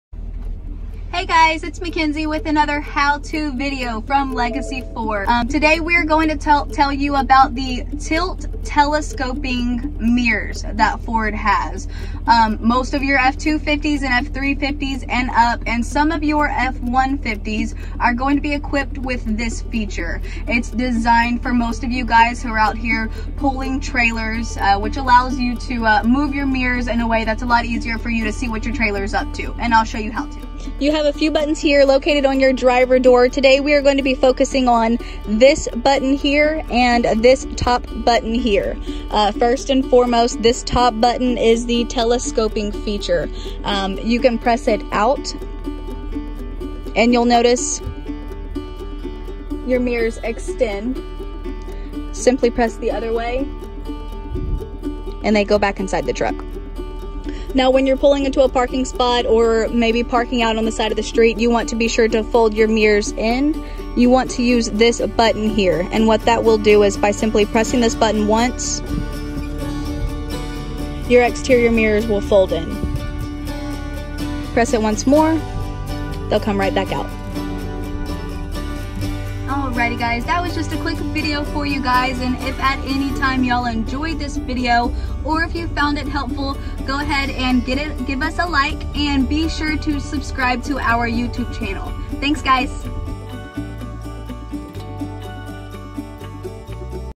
How To | Power Telescoping Mirrors
Makenzie shows you how to use your telescoping mirrors. Very East step by step process.

Legacy Ford TX loacated in Rosenberg TX. Family owned and operated dealership.